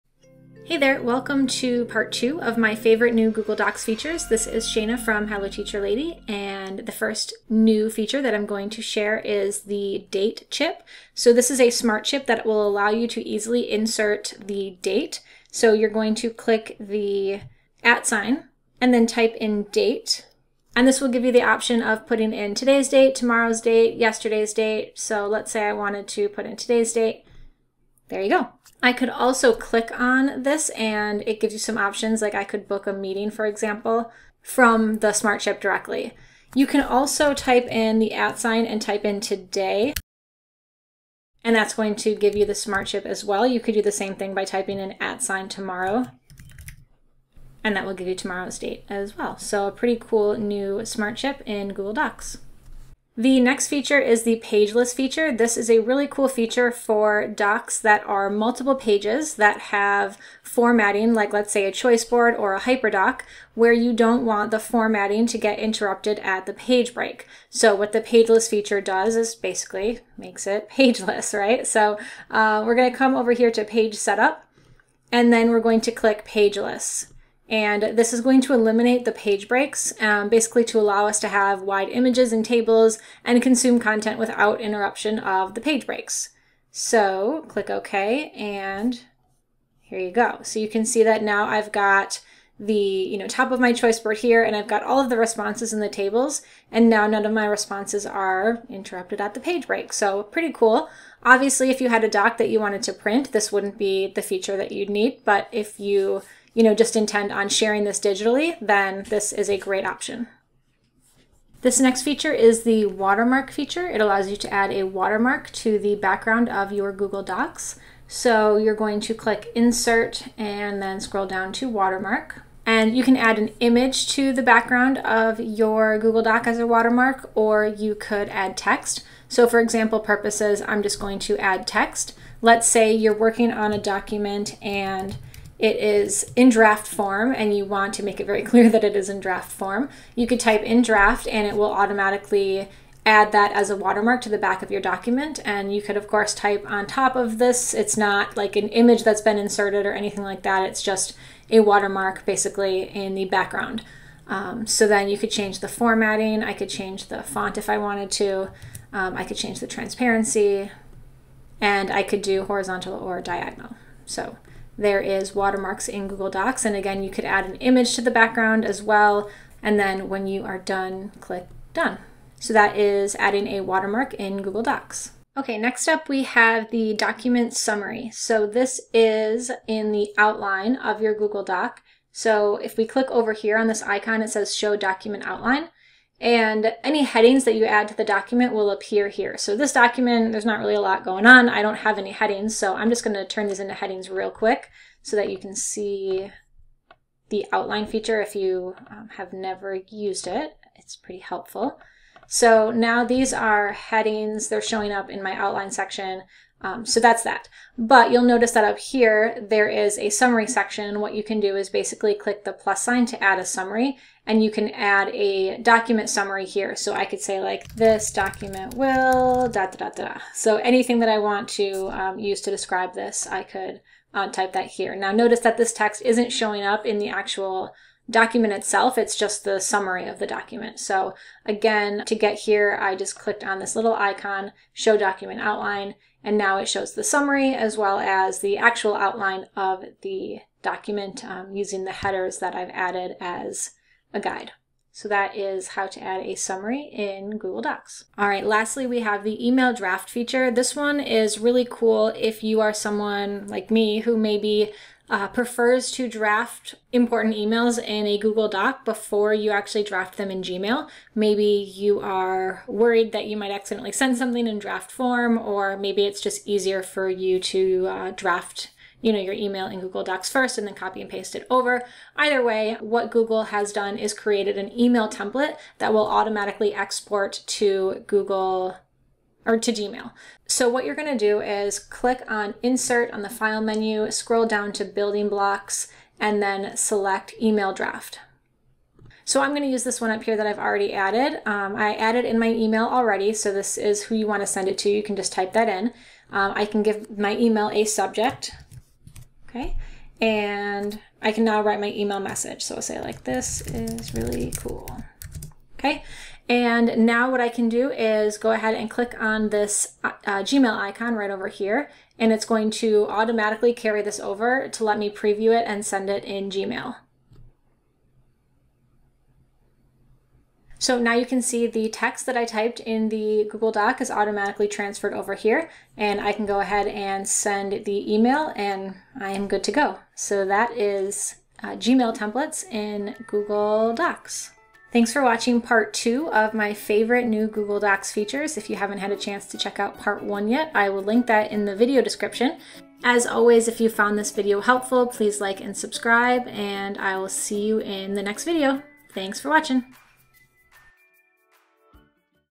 WHAT'S NEW IN GOOGLE DOCS?! 🎉 2022 Google Updates (PART 2)
Here's an overview 5 new Google Docs features.

⬇ TABLE OF CONTENTS ⬇
00:05 - Date Smartchips
00:53 - Pageless View
01:55 - Watermarks
02:58 - Document Summary
04:09 - Email Drafts